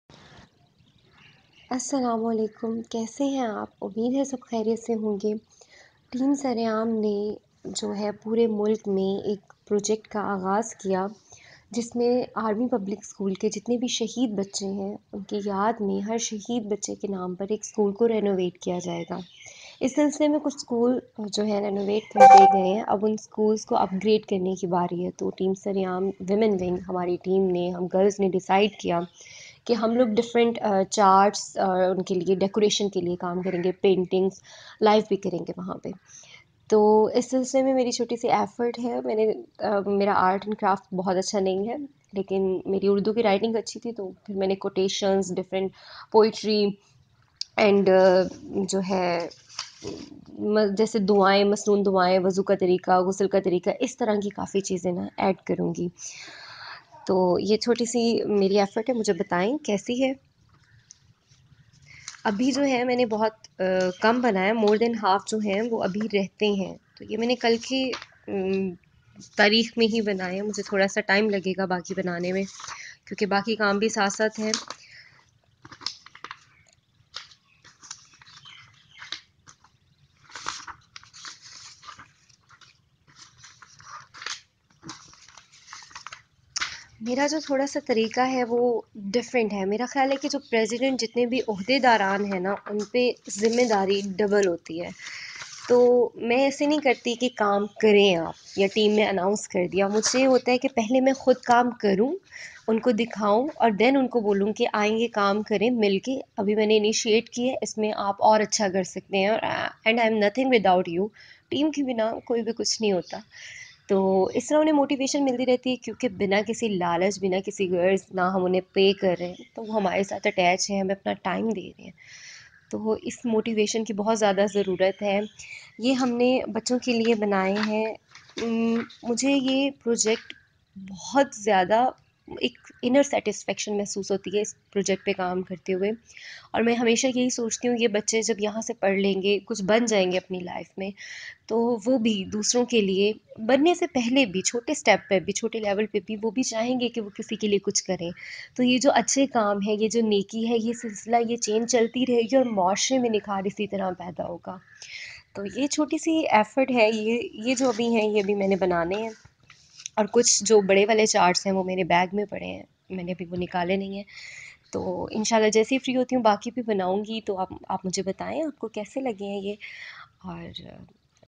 Preps for upgradation of schools in Memory of APS | Iqrar Ul Hassan initiative
Preparations for upgradation of schools in Memory of APS are on full swing, Team SareAam WW president Tahira Ehtesham sharing her views on subjected preparations.